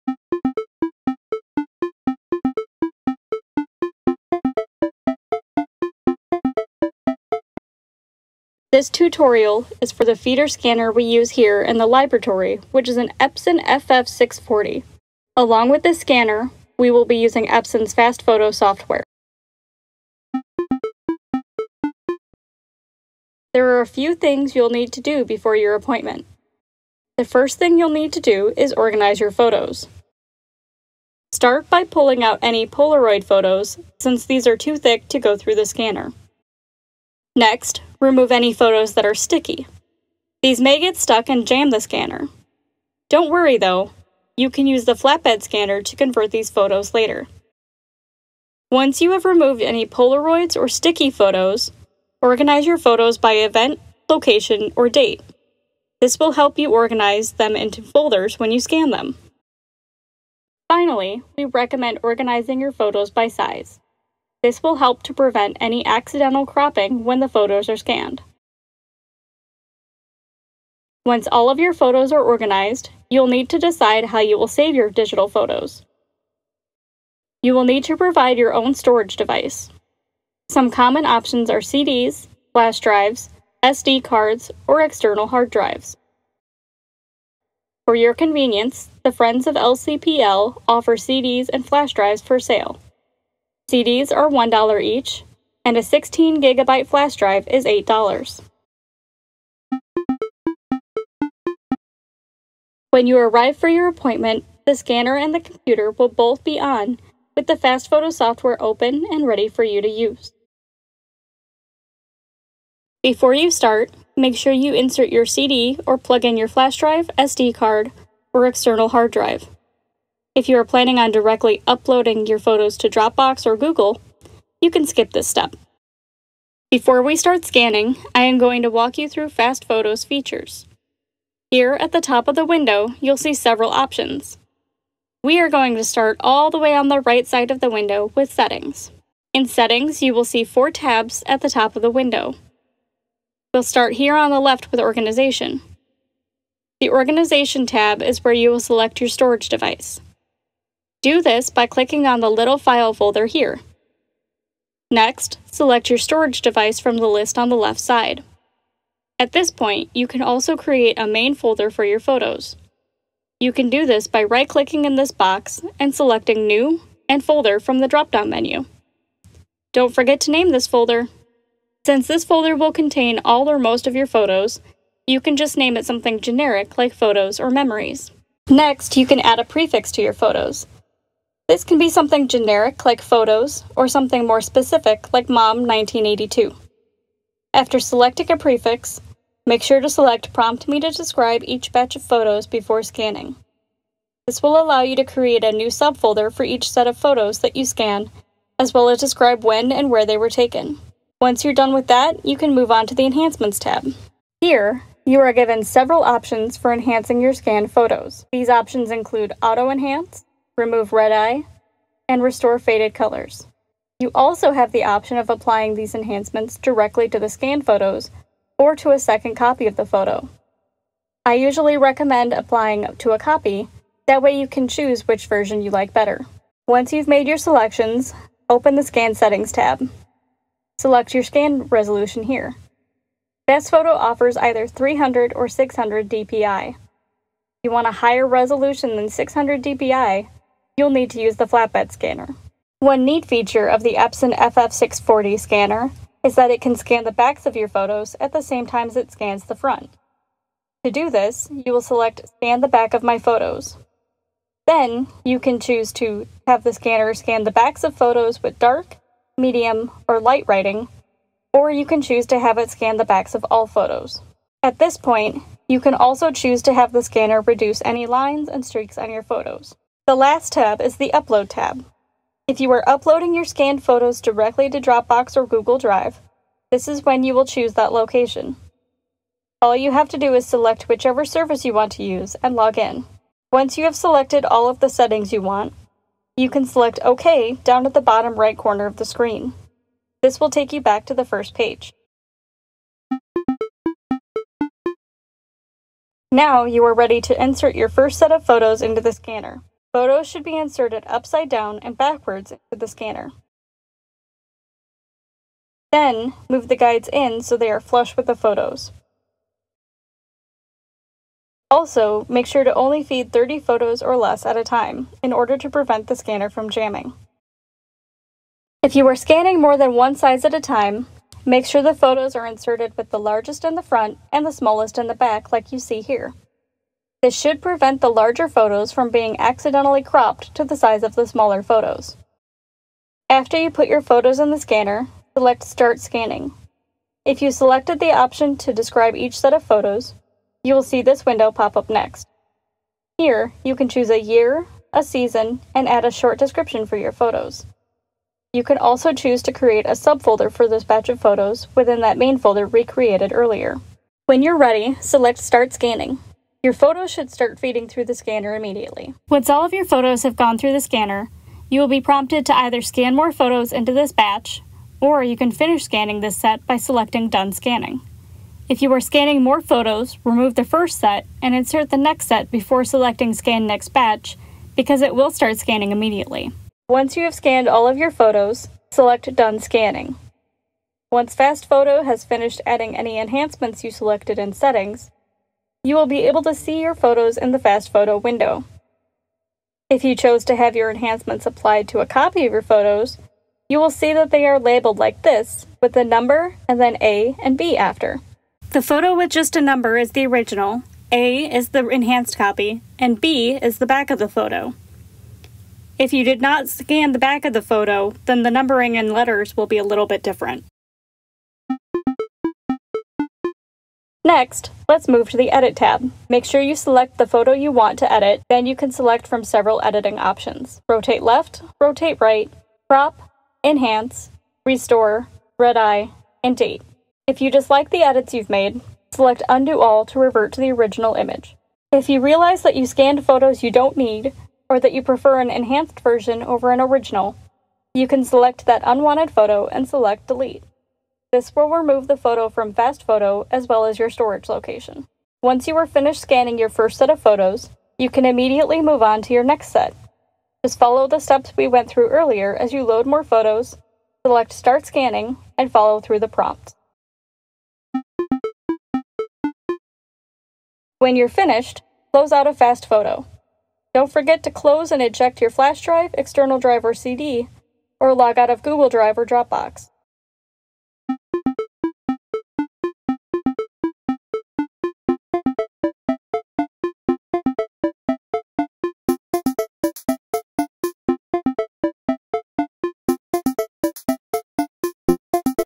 Epson FF-640 Feeder Scanner & FastFoto: Libratory Appointment Tutorial
Learn to use the Libratory's Epson FF-640 and FastFoto to quickly convert your physical photos into digital files! For more information on how to use this scanner, as well as other equipment in the Libratory, visit us

Intro - 0:00
Preparing for Your Appointment - 0:20
Getting Started - 1:39
Let’s Start Scanning - 4:53
Finishing Up - 7:38
More Questions? - 7:51